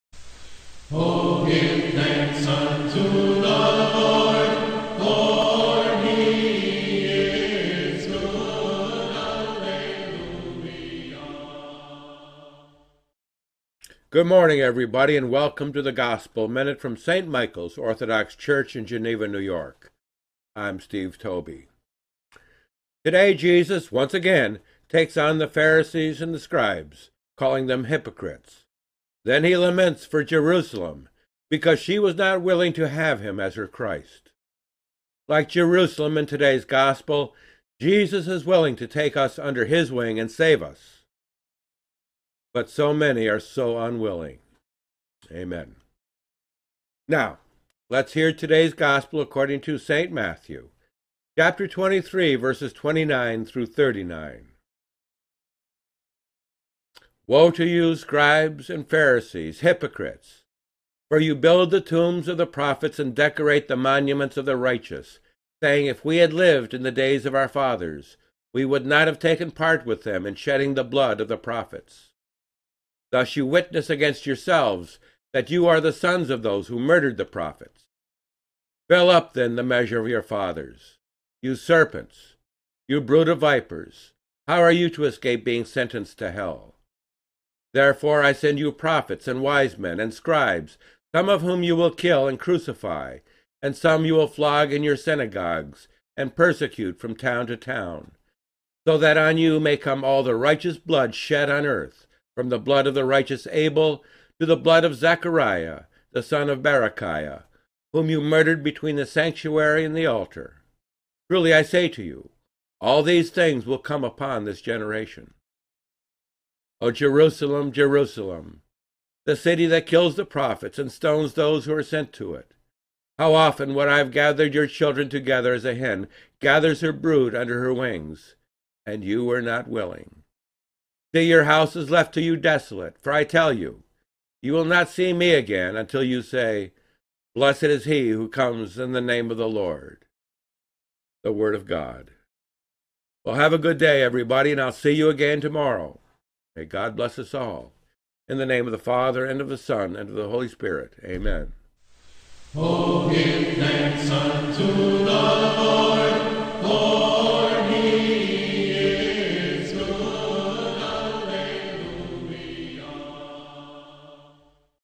"Blessed is he who comes in the name of the Lord." Aug 8
"Blessed is he who comes in the name of the Lord."
The Gospel Minute for August 8th, 2018
Today's Gospel Lesson is taken from St Matthew 23:29-39
Please share The Word Of God with your friends and families!